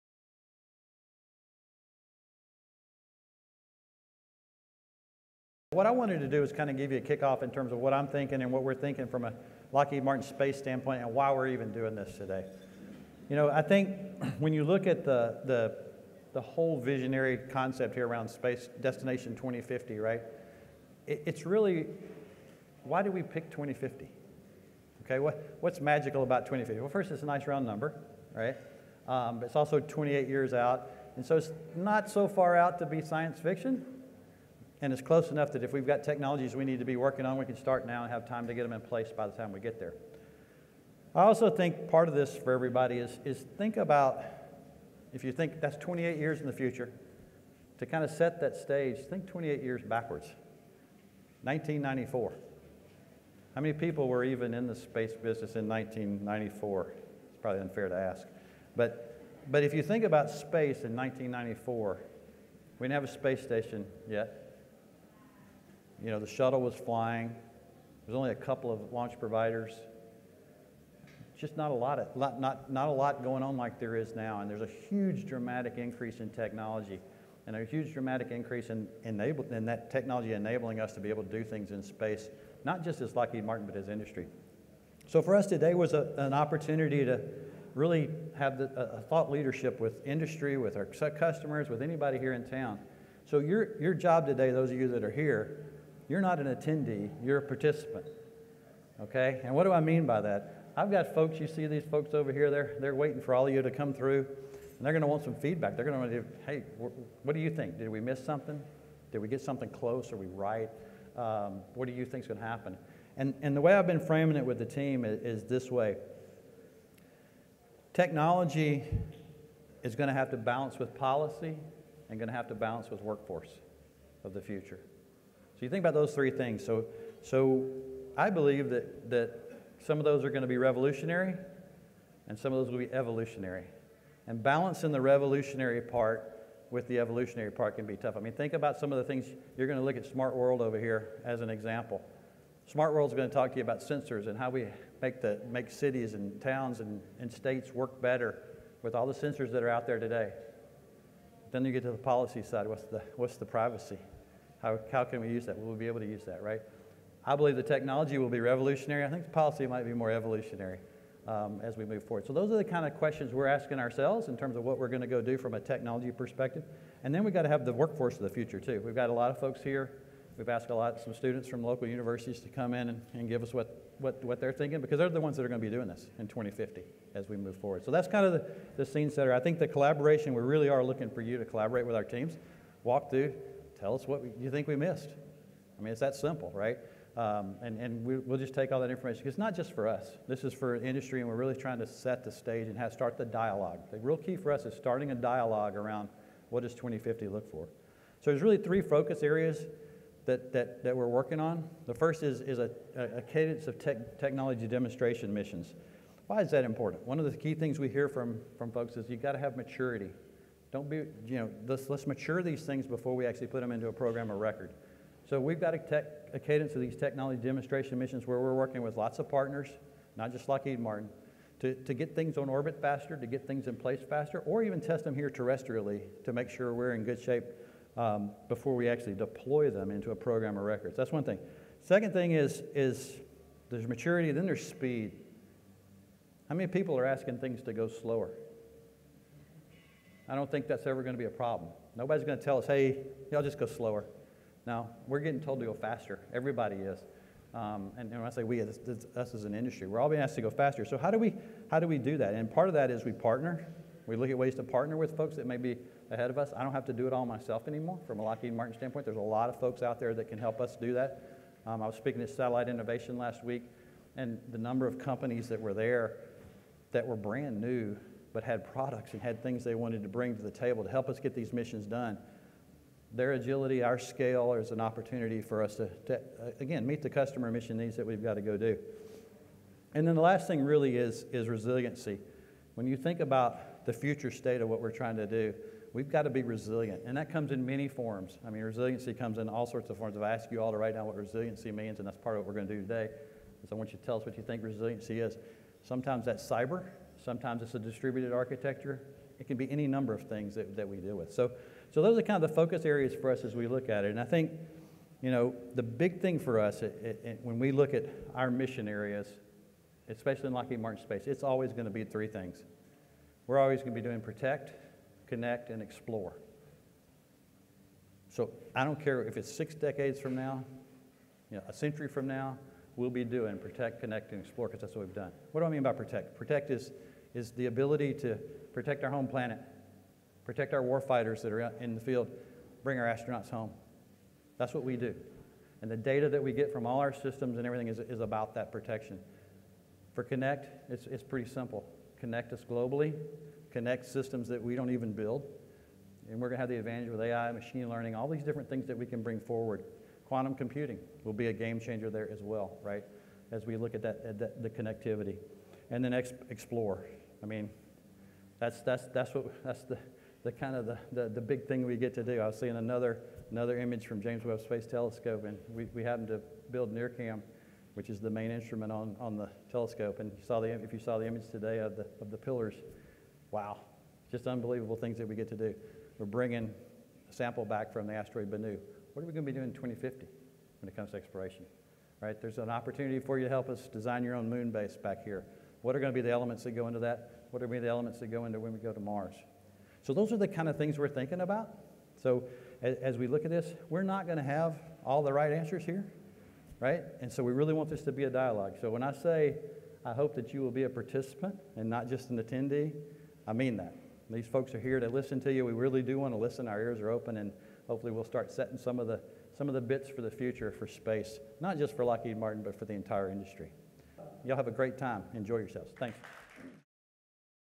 Destination: Space 2050 Live Event Opening Speech
Robert Lightfoot, EVP at Lockheed Martin Space delivers his message about how we're planning for the future.